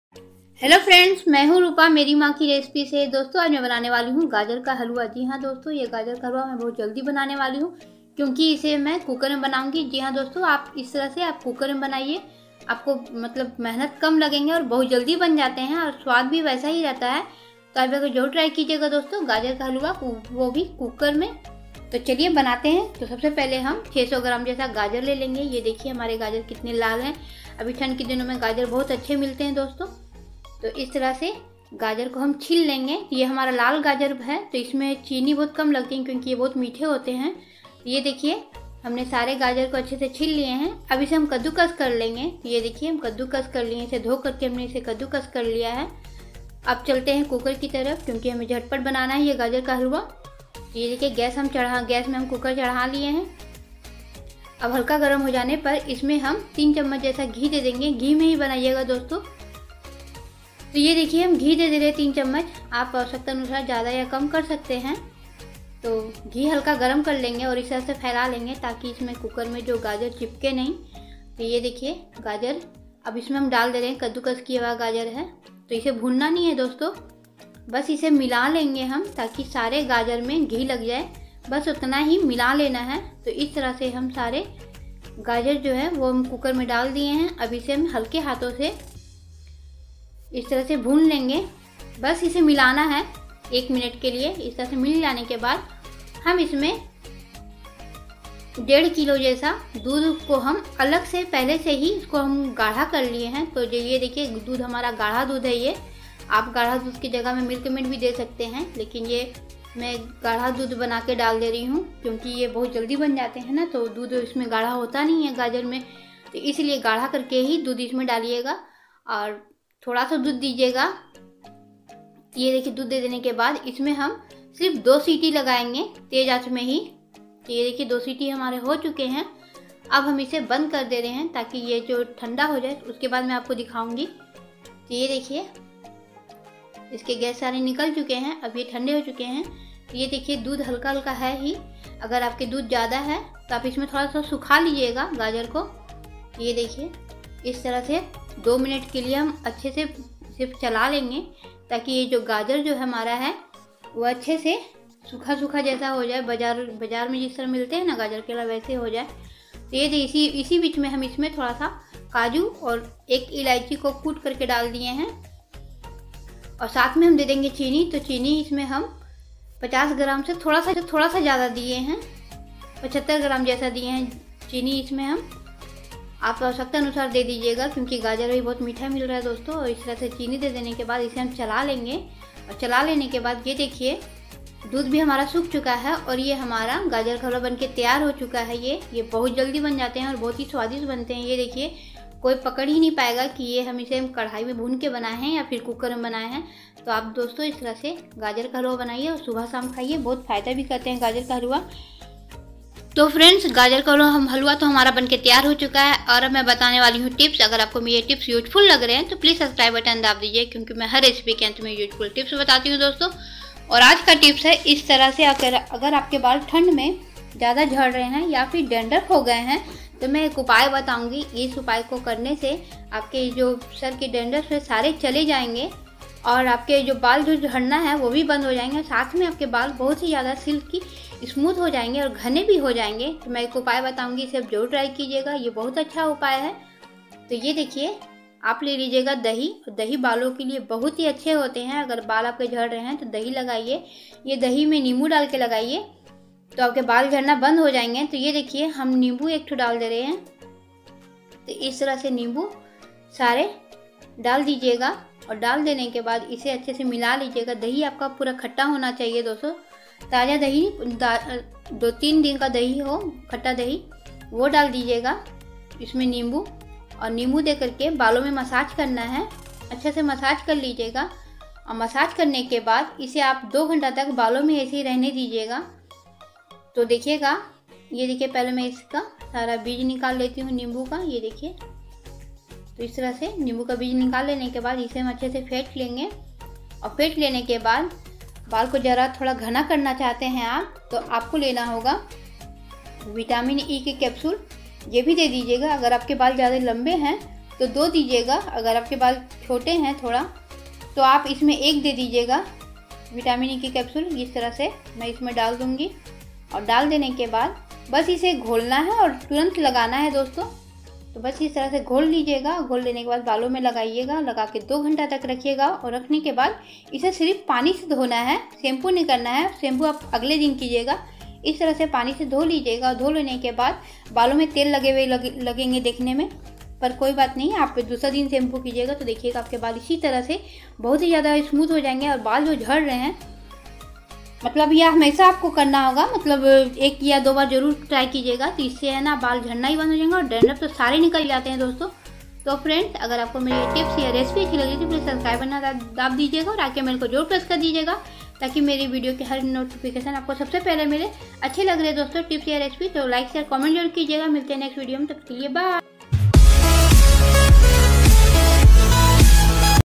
Cooker wala gajar ka halwa ▪︎वही टेस्ट मे बनाए कुकर मे गाजर क हलवा|टिप्स-सबके लिए एक ही हेयर पैक
Hi friends
How are you I hope you all are fine
I am Rupa and welcome back again in my channel Meri maa ki recipe.
today I am going to tech you how to make gajar ka halwa.

☞ swadisht khana.
☞ instant recipe.
☞ secret tips.
☞ easy recipes.
☞ new recipes.

○ INGREDIENTS WE NEED ARE :- ○
600 gram of carrot
100 gram of sugar
almonds
1.5 liter of milk

○ आवश्यक सामग्री हैं:- ○
600 ग्राम गाजर
100 ग्राम चीनी
बादाम
1.5 लीटर दूध

ok bye see you soon with more interesting recipe.

End